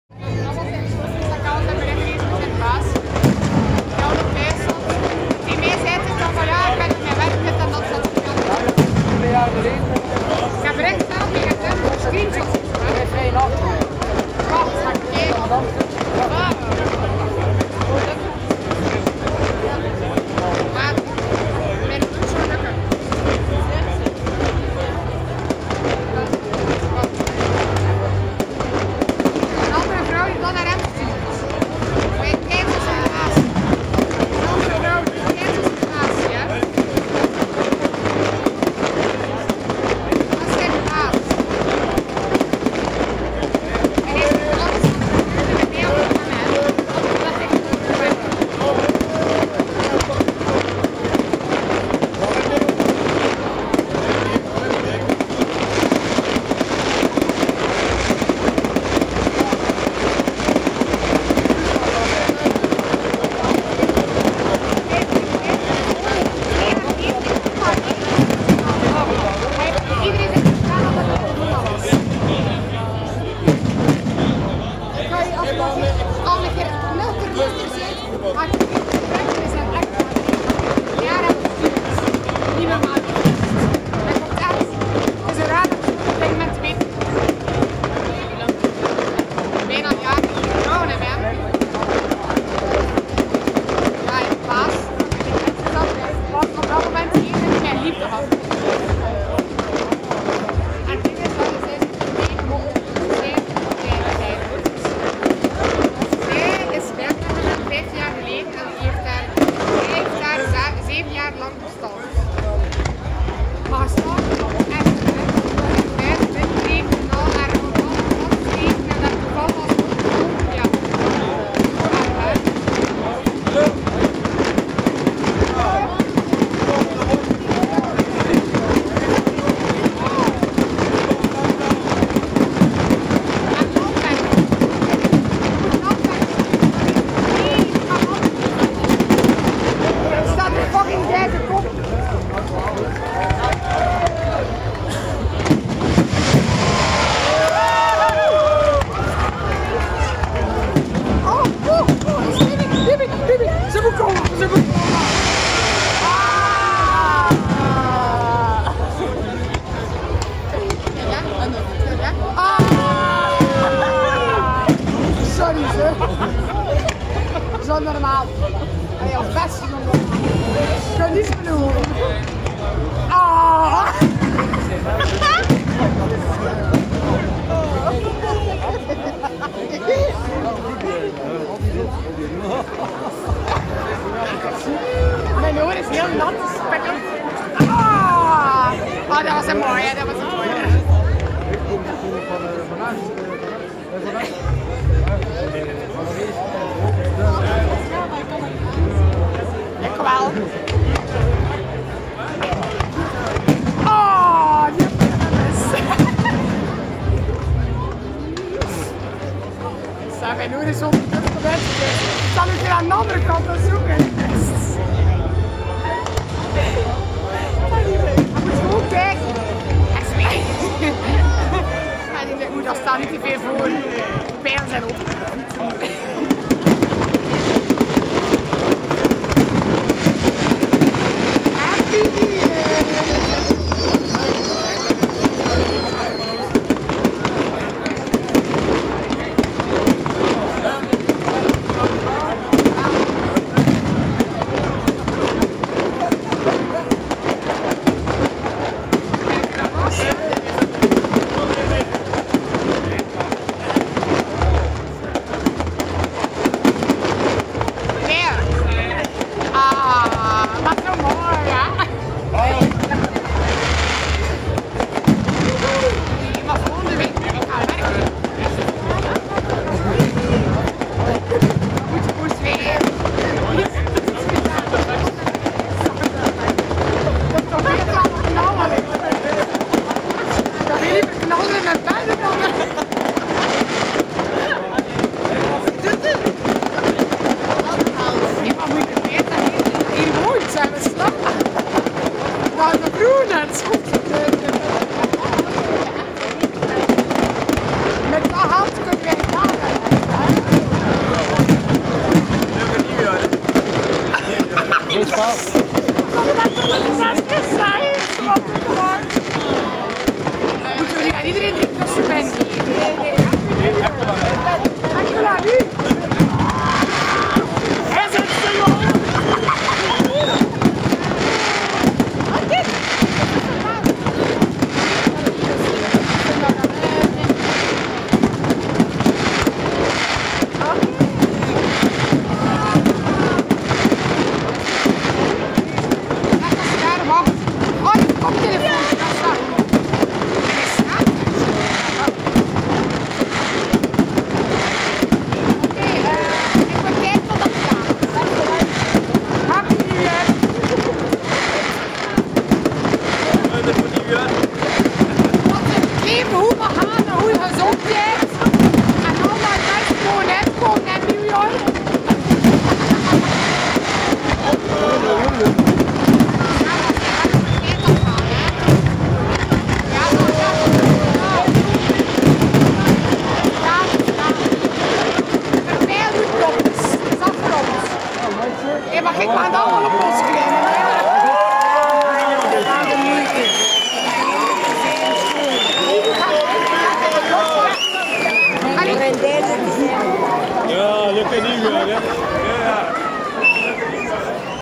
27/07/2022 firework at the kromme brug, belgium
op 27 juli aan de kromme brug was er vuurwerk

on july 27 at the kromme brug (literally translated to curved bridge) there were fireworks